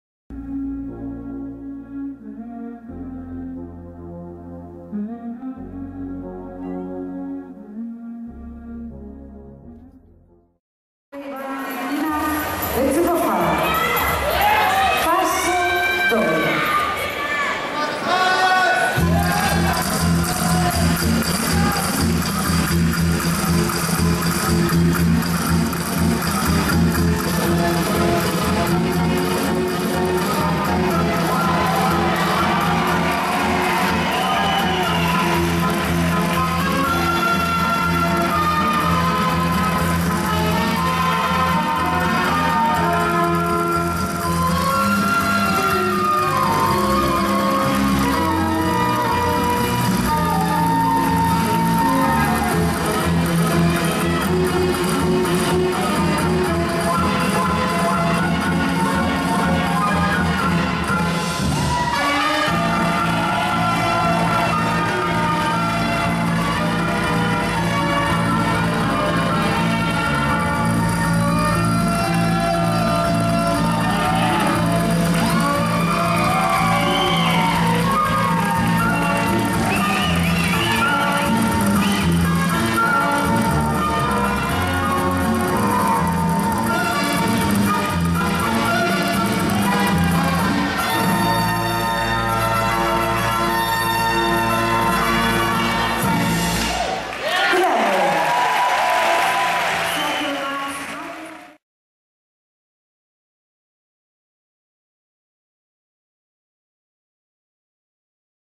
Cambrils 2012 - European Championship Latin - solo Paso Doble - Marius-Andrei Balan & Nina Bezzubova
Cambrils 2012, Final European Championship Latin, 8th April 2012, Cambrils/Spain.